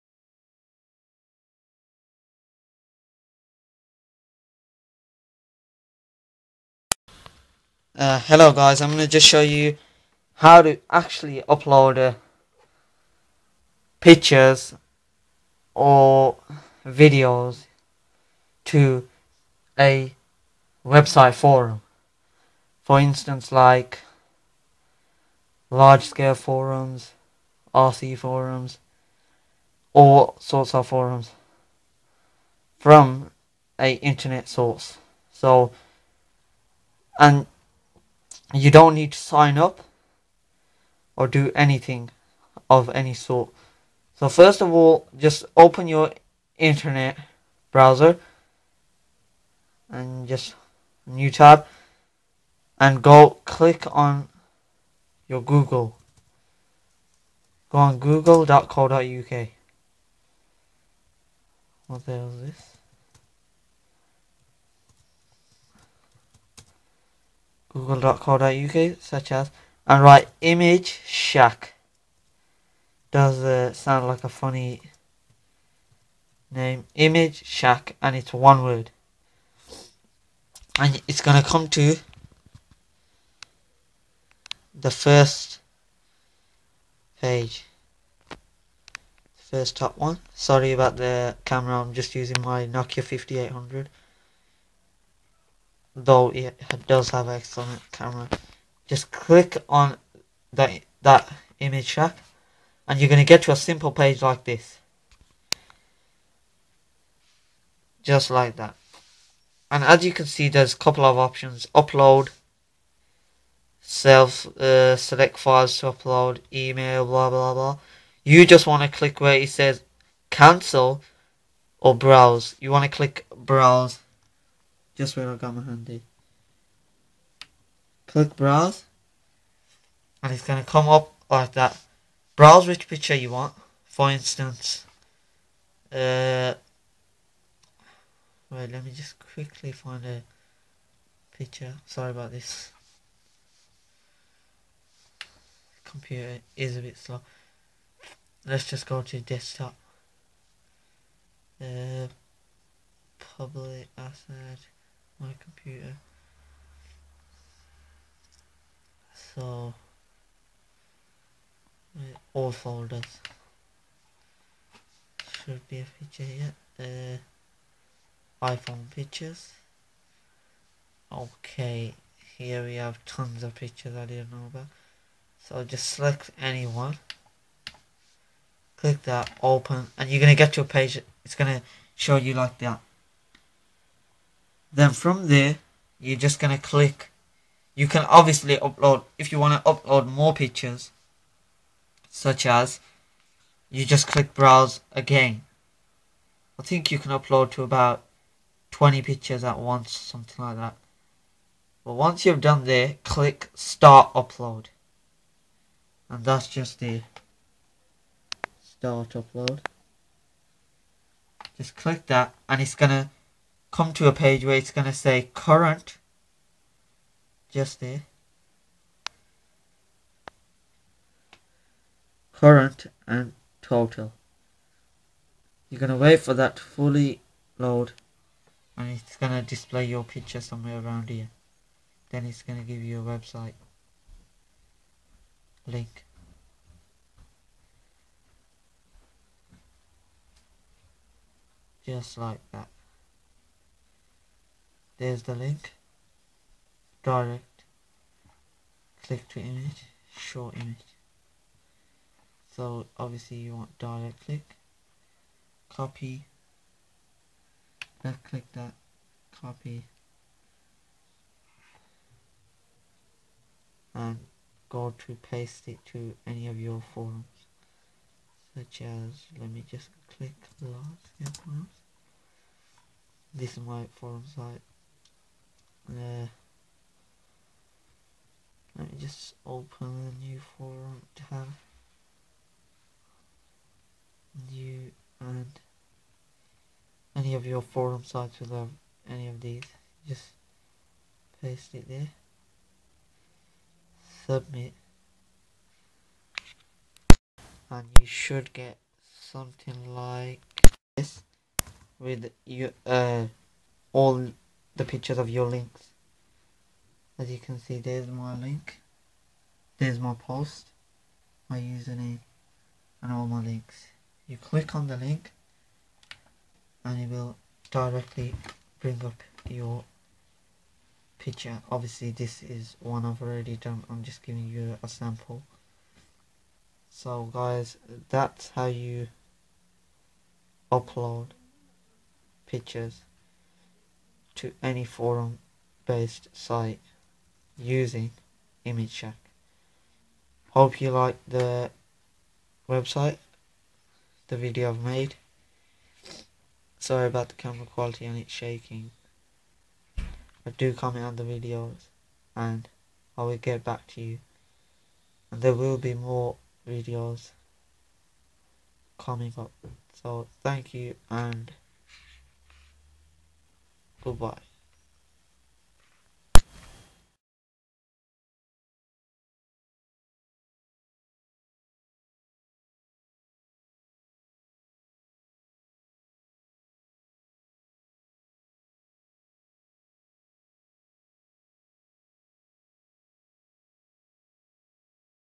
How to upload images to any website using imageshack in couple of easy steps
here i will be showing u how to upload images to any forum based website with image shack here is the link for image shack .